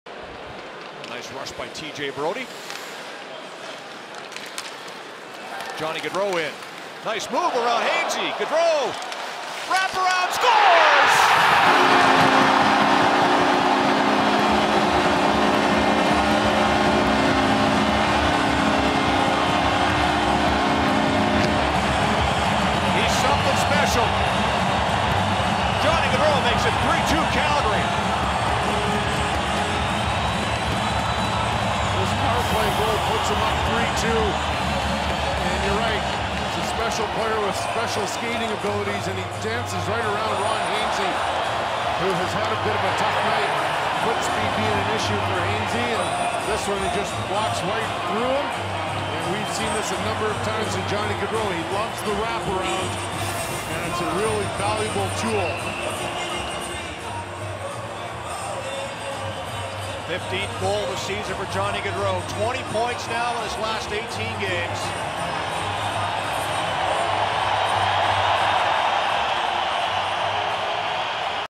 Gotta See It: Gaudreau does it all himself, weaves through traffic to score sensational goal
Calgary Flames forward Johnny Gaudreau pulls off an incredible individual effort to score a mesmerizing goal against Marc-Andre Fleury and the Pittsburgh Penguins.